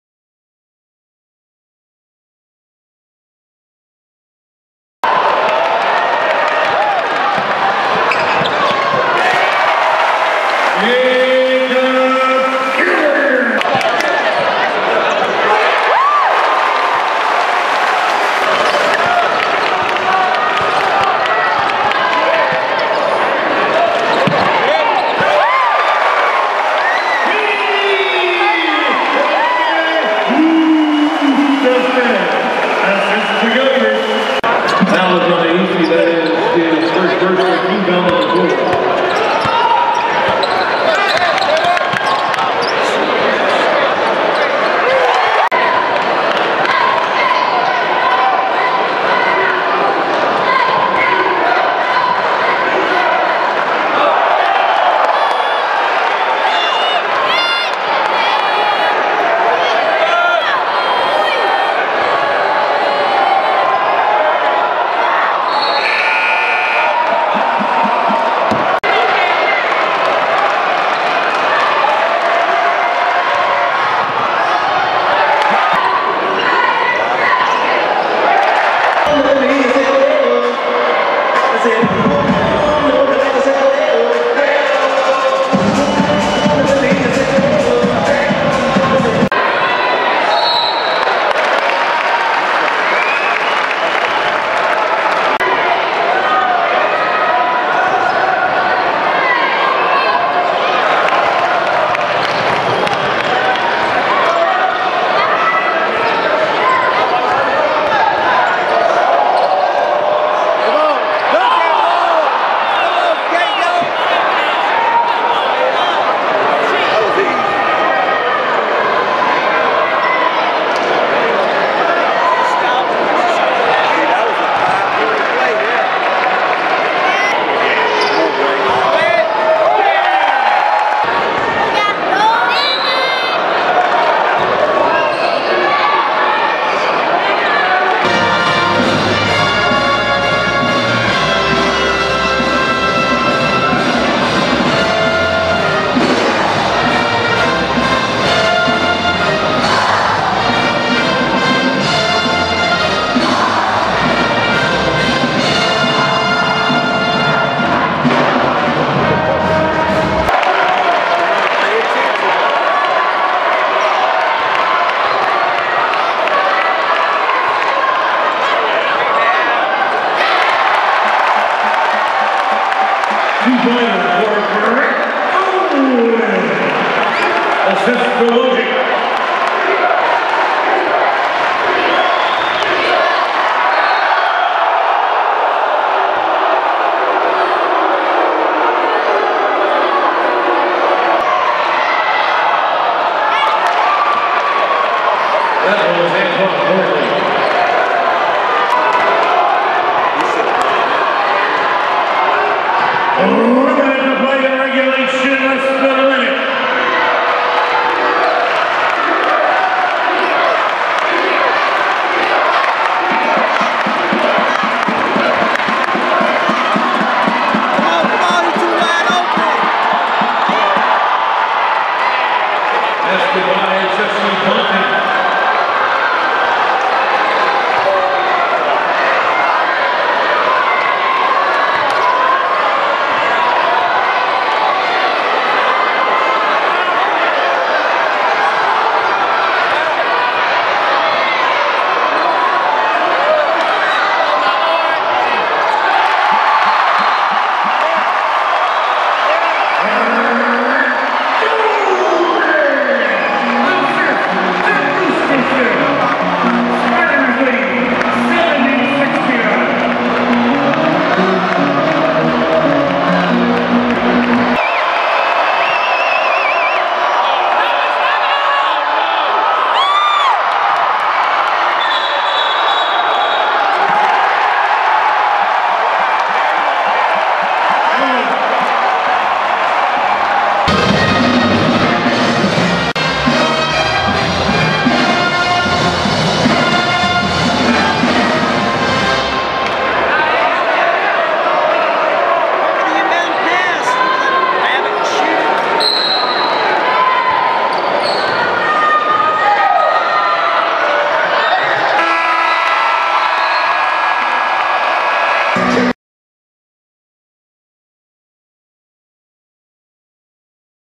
LMS Network Highlights: Mens College Basketball- Fordham Rams @ Richmond Spiders (2-20-19)
Video shot and edited by Robb Johnson. Richmond Spiders defeated the Fordham Rams 72-69 in a competitive basketball game (2-20-19)